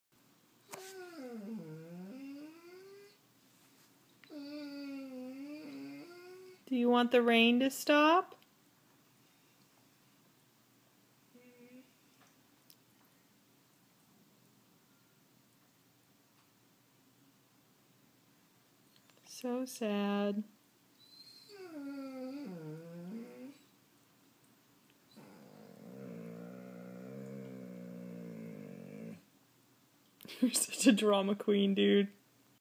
The woes of a borzoi
It's raining and he's moaning that he can't go outside.